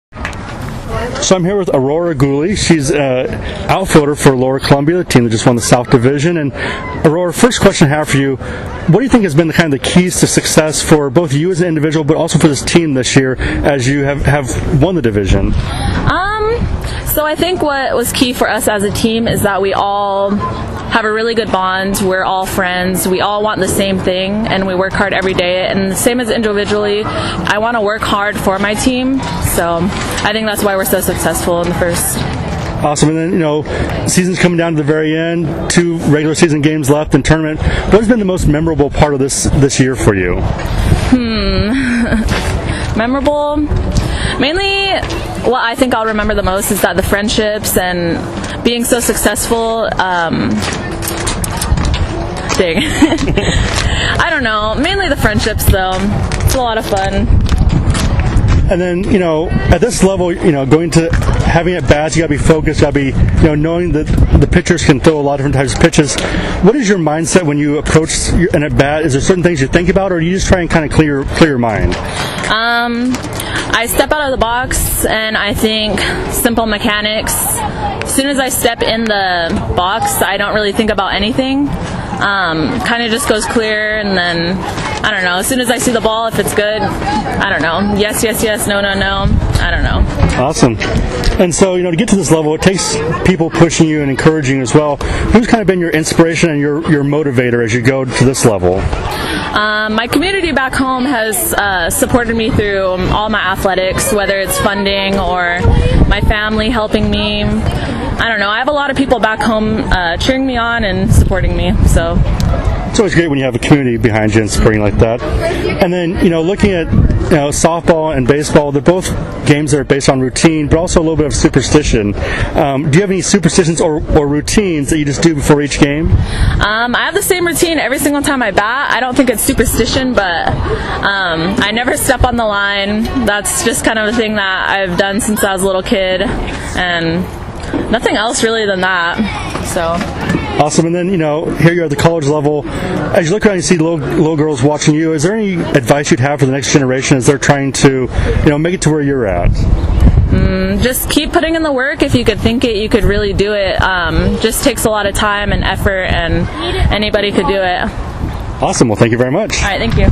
9 Inning Baseball Talk with guest Aurora Gouley.
9 Inning Baseball Talk with guest Aurora Gouley. Aurora is a Sophomore outfielder for LCC who recently clinched the 2018 NWAC South Division Title.